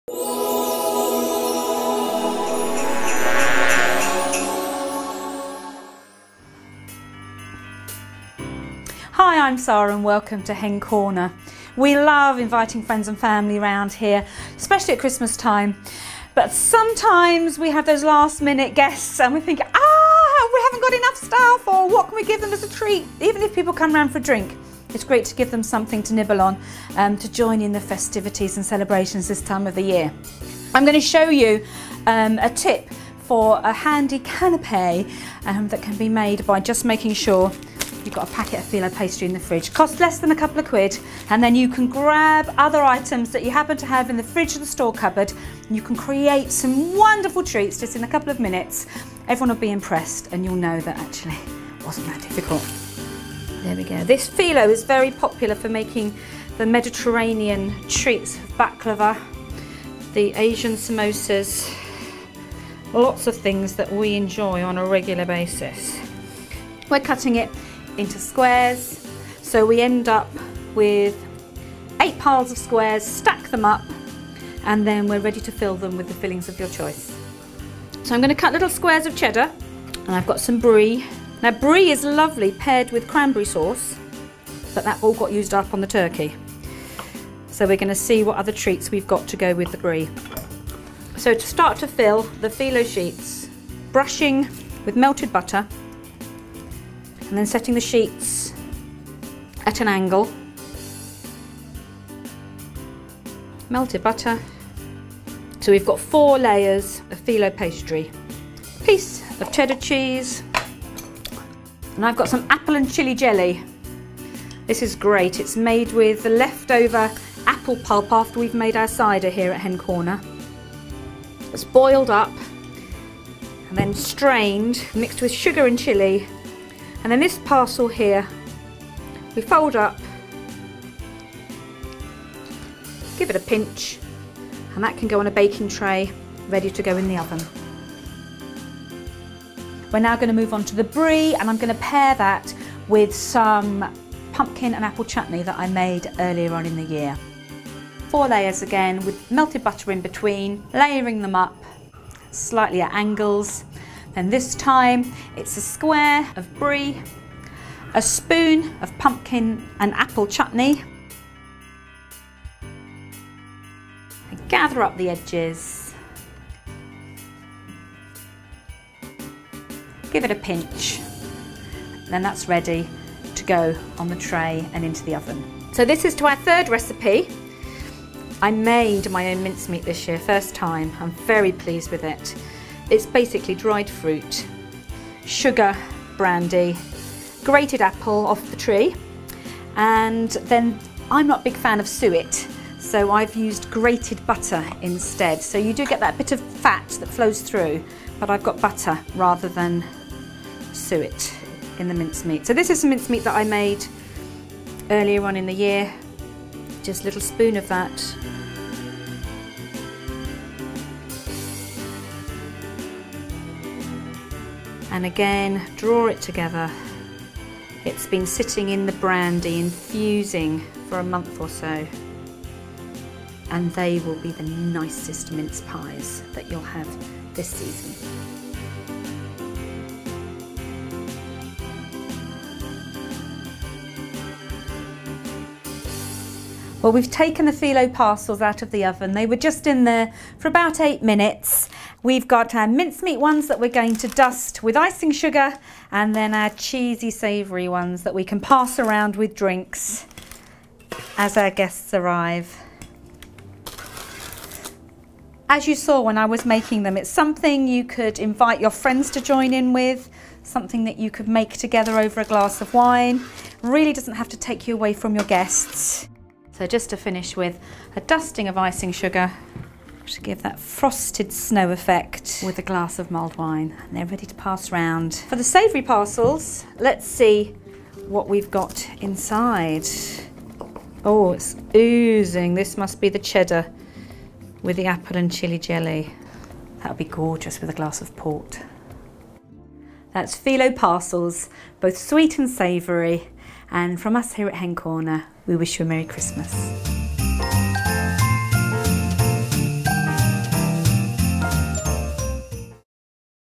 Hen Corner - Filo Parcels
How to make quick and easy canapes from fridge and larder staple ingredients. Using filo pastry, found in the supermarket chilled food section, Sara Ward of Hen Corner demonstrates an impressive treat to share with family and friends.